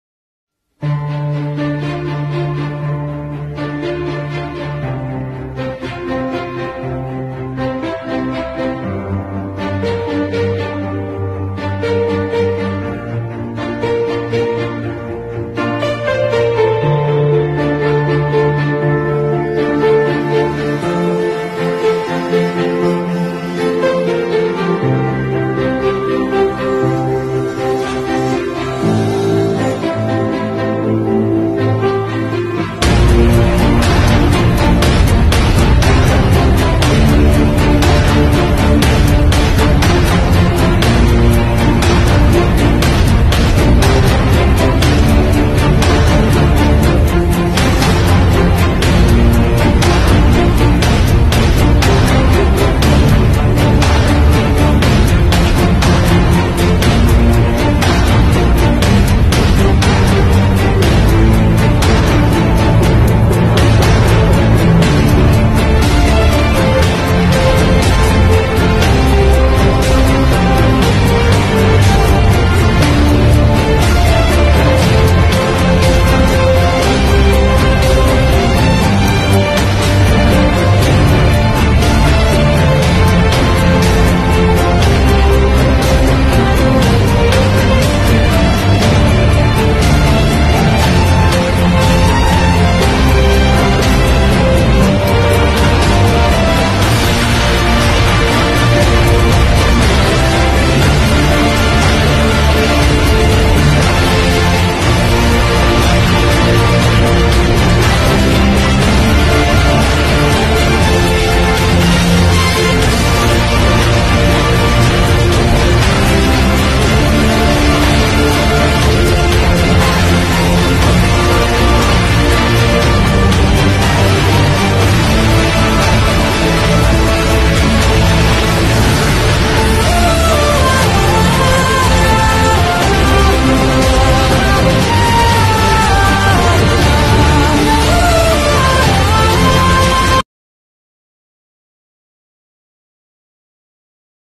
Brave woman
The brave woman who jumped from one plane to another in the air to changed the landing wheel of the plane in 1926.